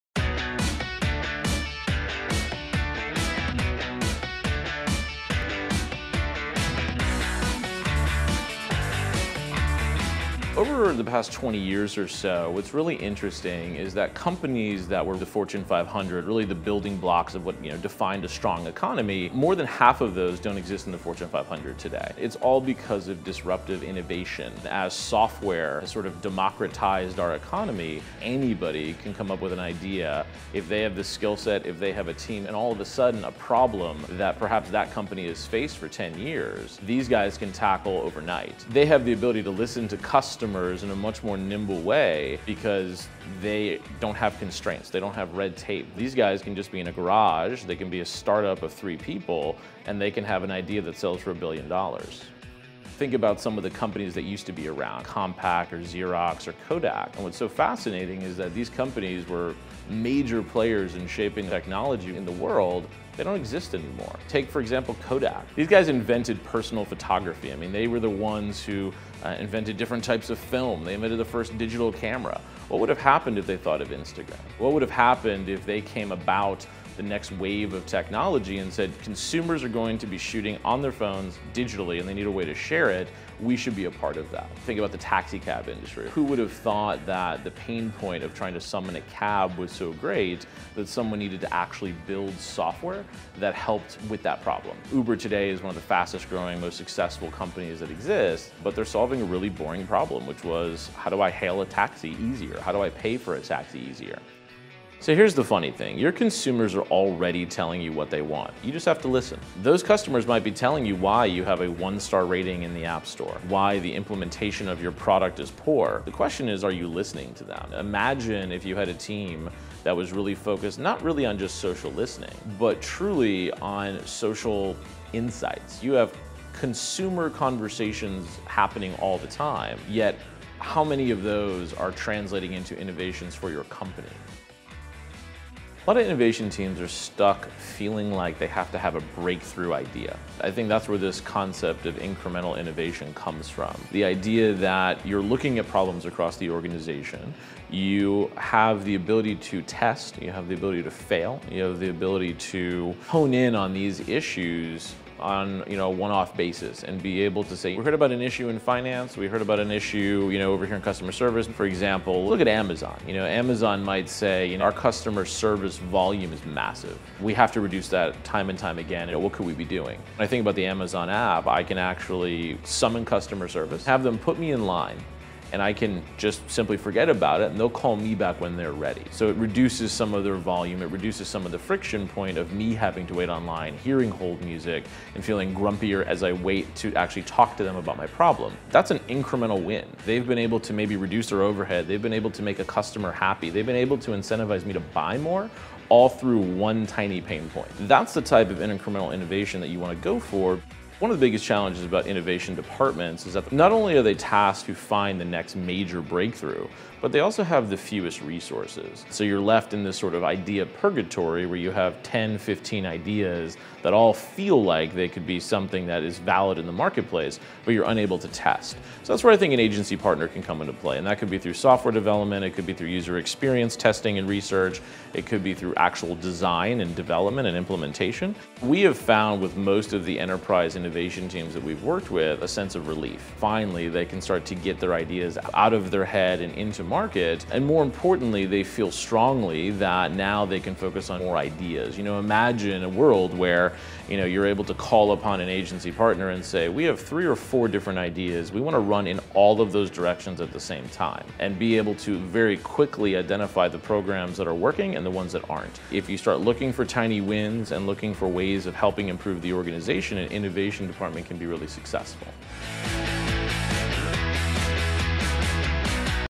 Finding Incremental Innovation Through Iteration - Three Five Two
Not all innovation requires breakthrough ideas or the disruption of an entire industry – sometimes it just requires corporate innovators to think about emerging, boring problems in a new way. If enterprises can learn to listen to the right customers, they can tackle innovation in an incremental way that guarantees success.